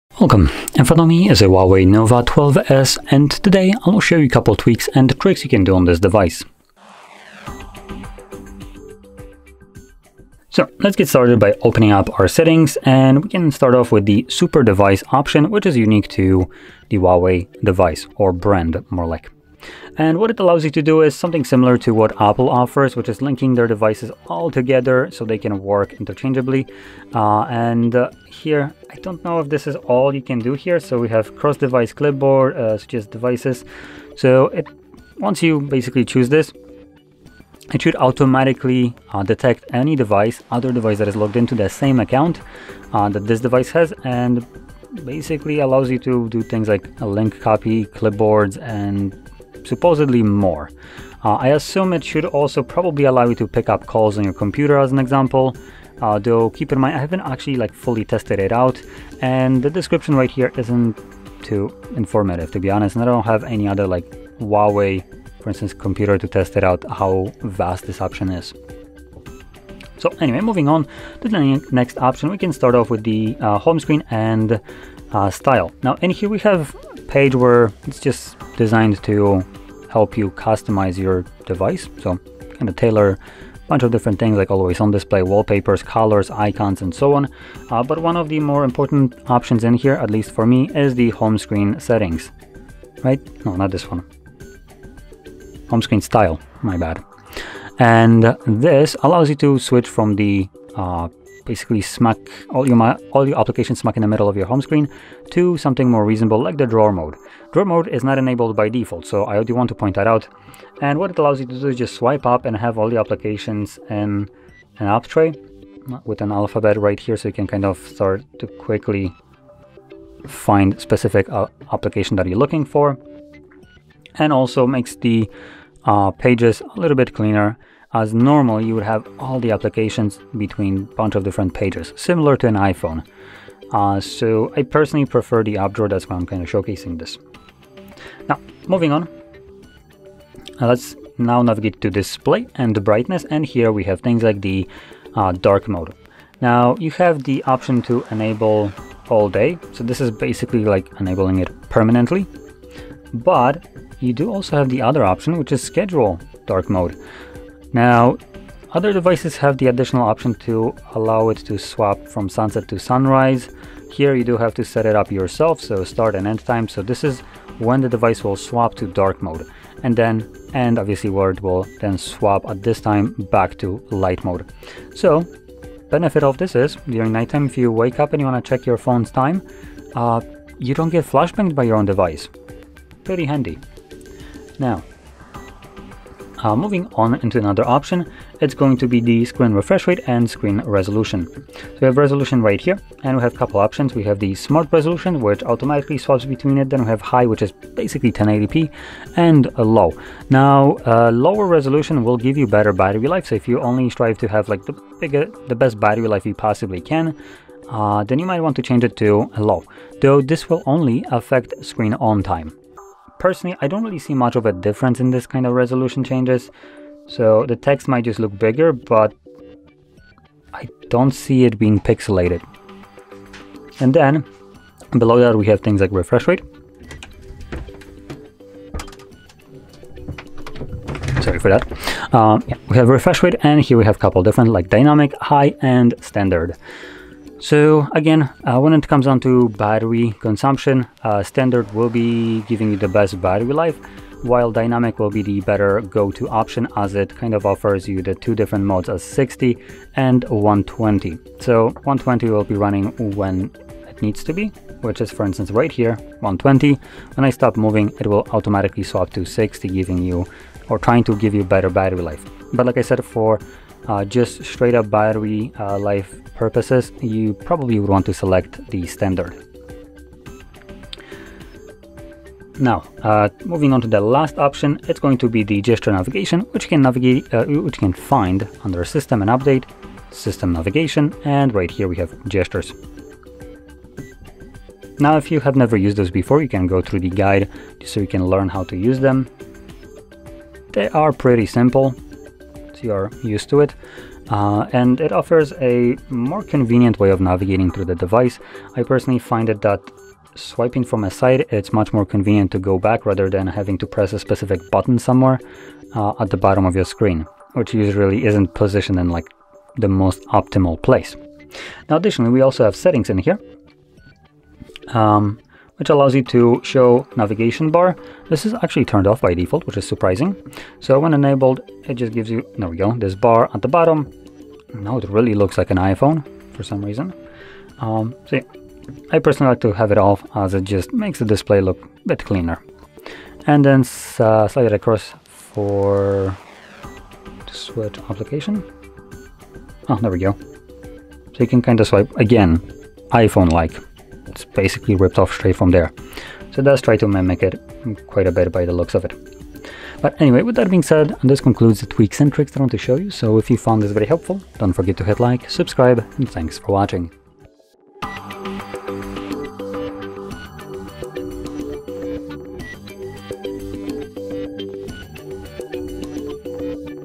Tips & Tricks for HUAWEI Nova 12s
Find out more:
This tutorial is your ultimate guide to uncovering the hidden features and maximizing the potential of your HUAWEI Nova 12s. From optimizing performance to mastering camera settings, these tips and tricks will help you make the most out of your device. Whether you're a seasoned user or new to the Nova 12s, follow along to discover how to enhance your smartphone experience.

What are some lesser-known features of the HUAWEI Nova 12s that users might overlook?
Can you share any tips for improving battery life on the Nova 12s?
How do I customize the user interface and personalize my Nova 12s experience?
Are there any shortcuts or gestures that can help me navigate my Nova 12s more efficiently?
What are some security tips or features to keep my Nova 12s safe from threats?